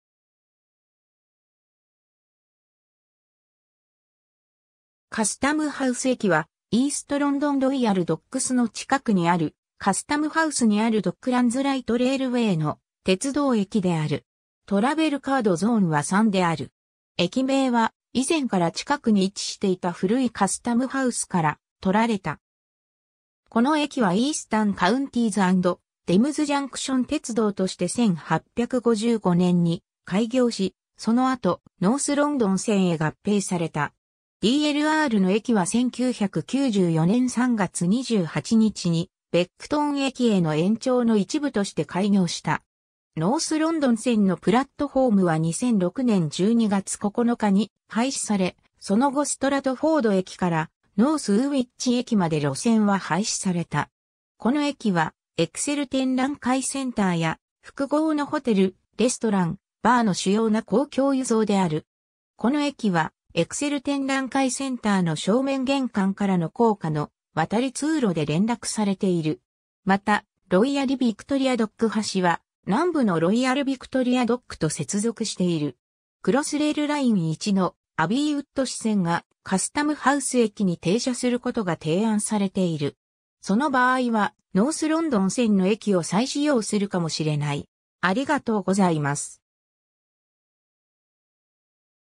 カスタム・ハウス駅
ドックランズ・ライト・レイルウェイの駅
カスタム・ハウス駅（Custom House station）は、イースト・ロンドンロイヤル・ドックスの近くにあるカスタム・ハウスにあるドックランズ・ライト・レイルウェイ(DLR)の鉄道駅である。
トラベルカード・ゾーンは3である。
駅名は以前から近くに位置していた古いカスタム・ハウスから取られた。
 この駅はイースタン・カウンティーズ・アンド・テムズ・ジャンクション鉄道として1855年に開業し、その後ノース・ロンドン線へ合併された。
DLRの駅は1994年3月28日にベックトン駅への延長の一部として開業した。
ノース・ロンドン線のプラットホームは2006年12月9日に廃止され、その後ストラトフォード駅からノース・ウーウィッチ駅まで路線は廃止された。
 この駅は、エクセル展覧会センターや複合のホテル、レストラン、バーの主要な公共輸送である。
この駅はエクセル展覧会センターの正面玄関からの高架の渡り通路で連絡されている。
また、ロイヤリ・ヴィクトリア・ドック橋は南部のロイヤル・ヴィクトリア・ドックと接続している。
 クロスレイルライン1のアビー・ウッド支線がカスタム・ハウス駅に停車することが提案されている。
その場合はノース・ロンドン線の駅を再使用するかもしれない。